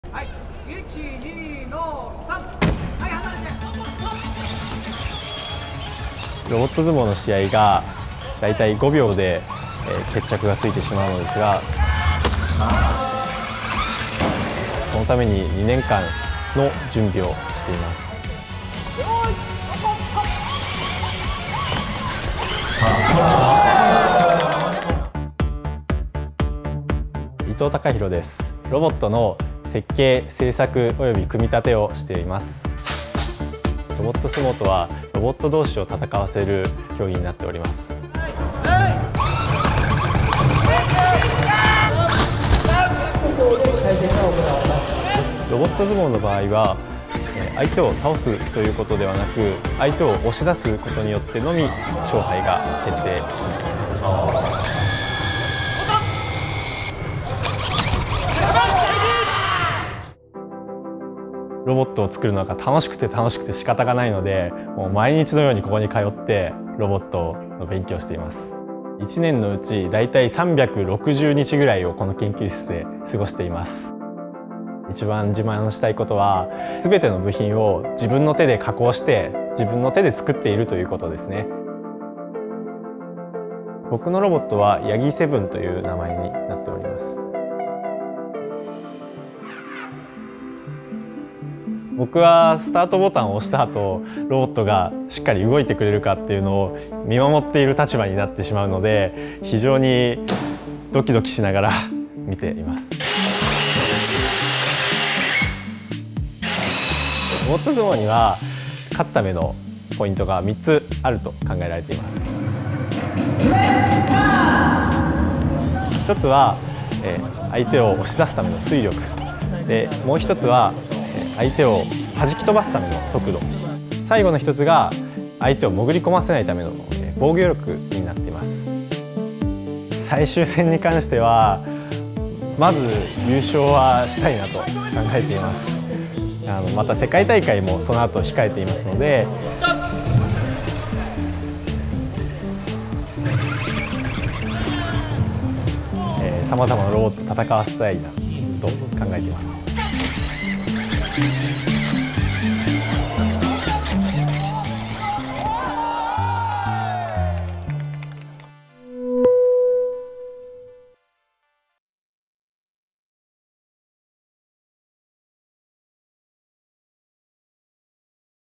The Freakishly Fast World of Robot Sumo
Welcome to the lightning-fast world of robot sumo, where two robots battle it out for mechanical supremacy. But, keep your eyes peeled—robots that take years to build can be decimated within mere seconds. Robot sumo has developed into a furiously competitive sport in Japan, with participants spending years building their autonomous machines. Just ask Takahiro Ito, a robot designer and builder who spends 360 days a year working on his craft. It's a demanding sport that requiring extreme dedication, all for a few seconds of glory.